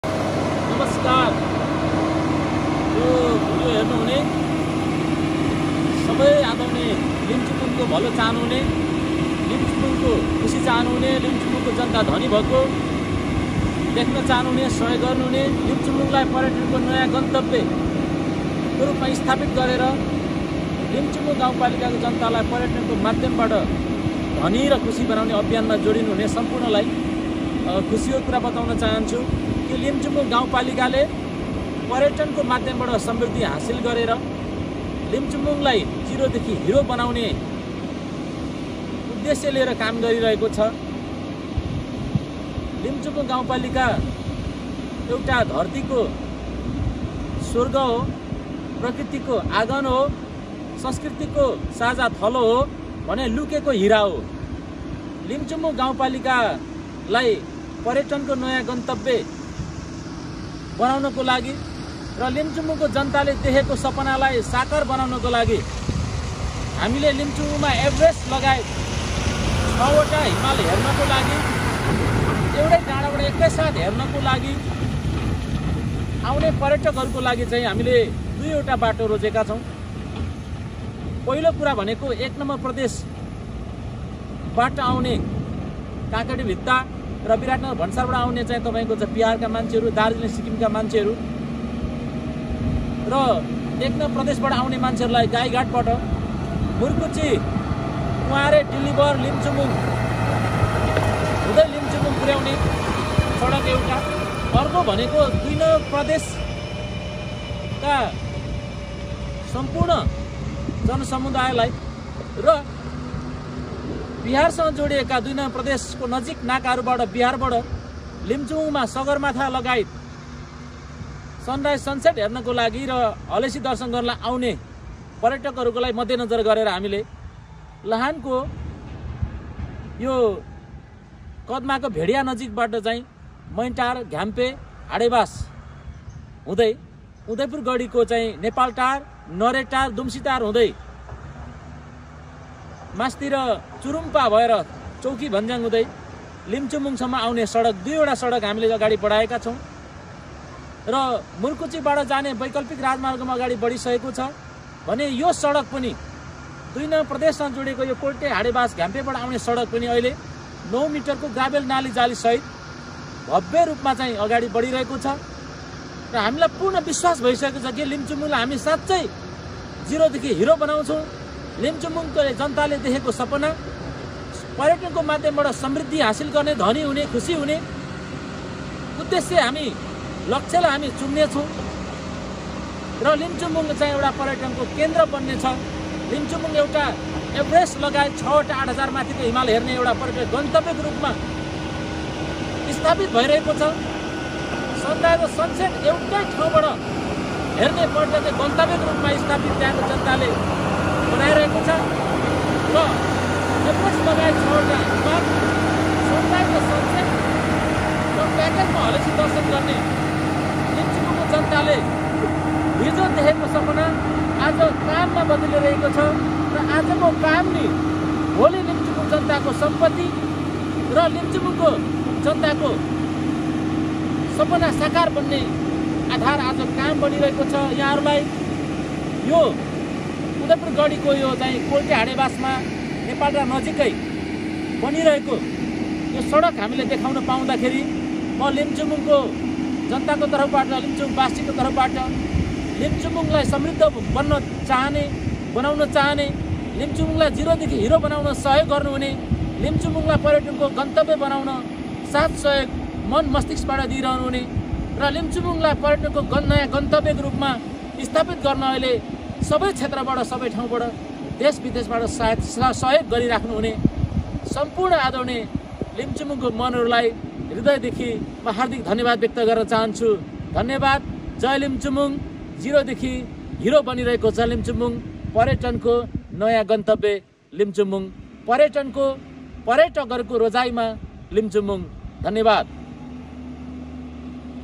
सगरमाथा नजिकबाट हेर्न लिम्चुङबङ आउनुहोस 🙏
सगरमाथा हेर्न जाने सडक धमाधम बन्दै ✌️

लिम्चुङबुङबाट सगरमाथ हेर्न सबै भन्दा नजिक र राम्रो देखिने भएकोले नेपालको मधेश प्रदेश र छिमेकी मुलुक भारतको विहार राज्यबाट सगरमाथा हेर्न लिम्चुङबुङ भ्रमणमा आउने पर्यटकहरुको लागि सहज र सबै भन्दा छिटो सडक अन्तरगत पृथिवी राजमार्गको कद्माचोक सिसुवारी देखि कोल्टे मईन्टार-घ्याम्पेहाडेवास-नेपालटार नगरेटादुम्सिटार हुदै चुरुमपाचौकिभञ्ज्याङ लिम्चुङबुङ सडक बनिरहेको छ।
परिवर्तनको आवाज लिम्चुङबुङ 🙋‍♂️